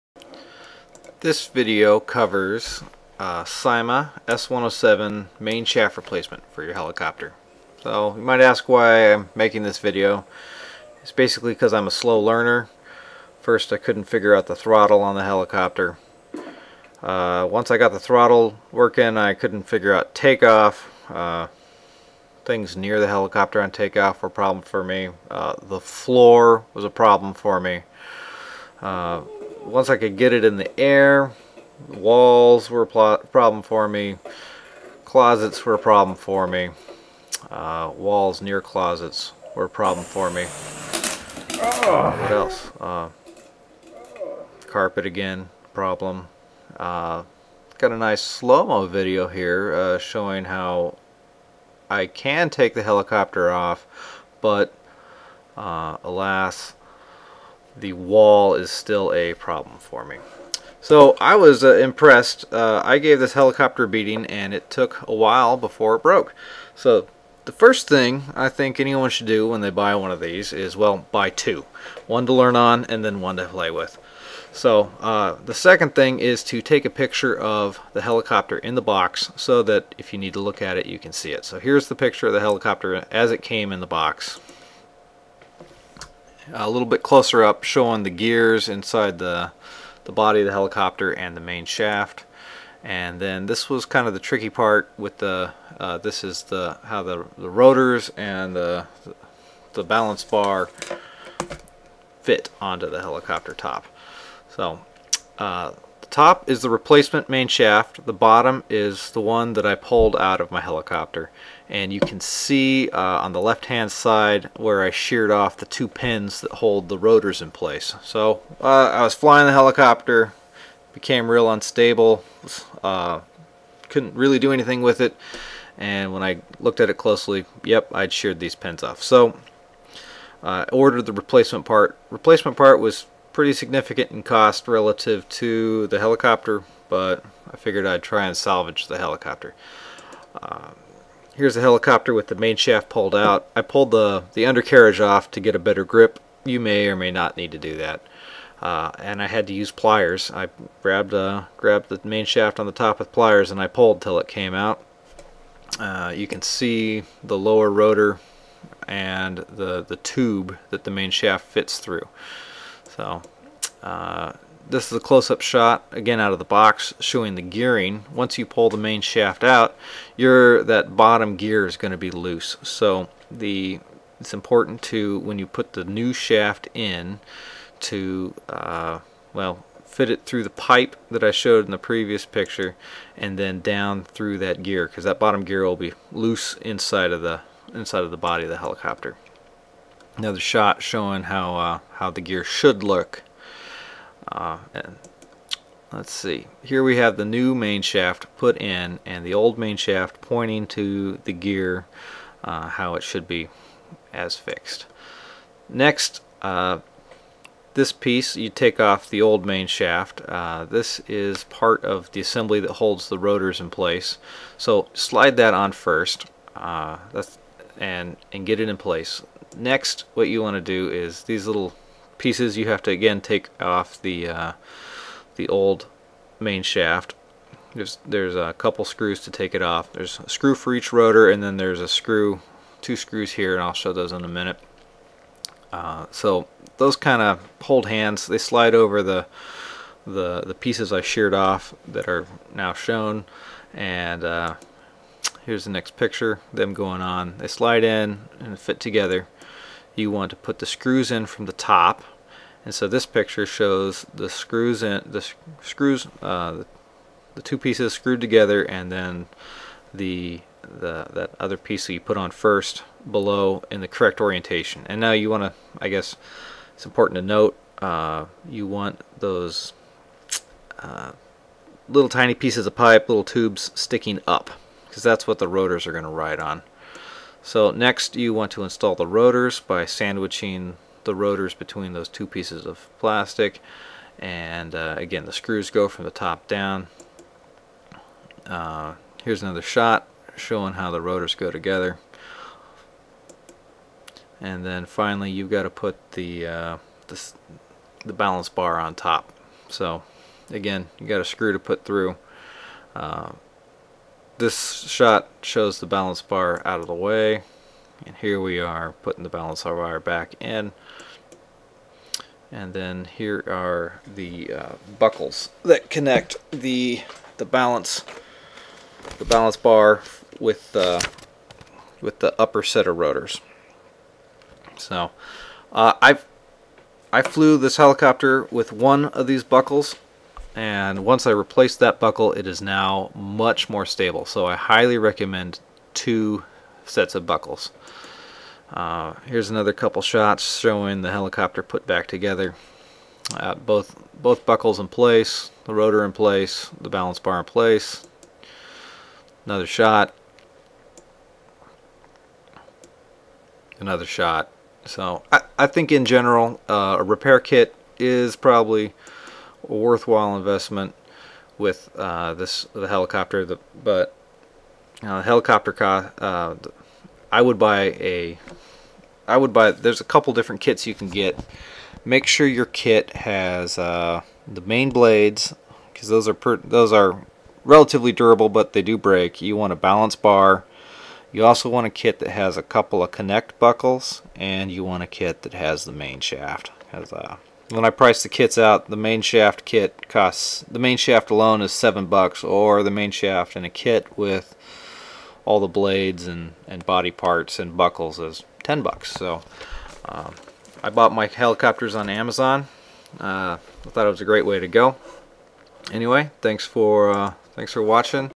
Syma S107 Main Shaft Replacement
Shows reassembly of a Syma S107 S107G RC Helicopter with a new main shaft. Part S107G-13 main shaft.